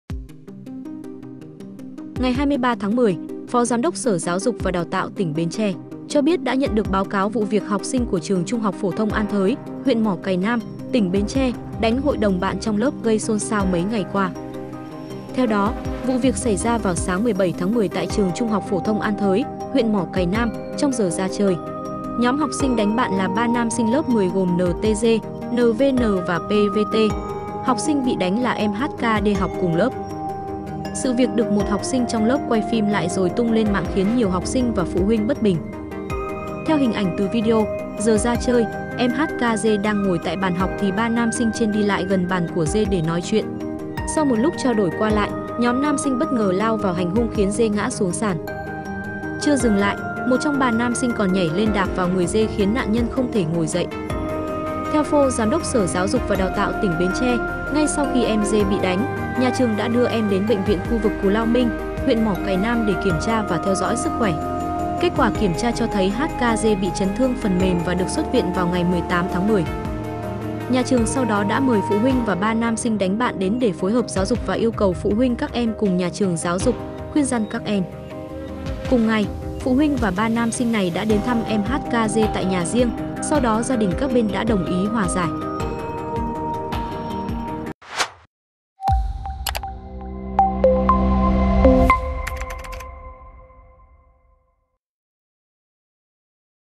Nam sinh lớp 10 bị đánh hội đồng ngay trong lớp phải nhập viện
Trong giờ ra chơi, ba nam sinh lớp 10 tại Bến Tre xúm lại đánh hội đồng một bạn nam cùng lớp khiến nạn nhân phải nhập viện sau đó.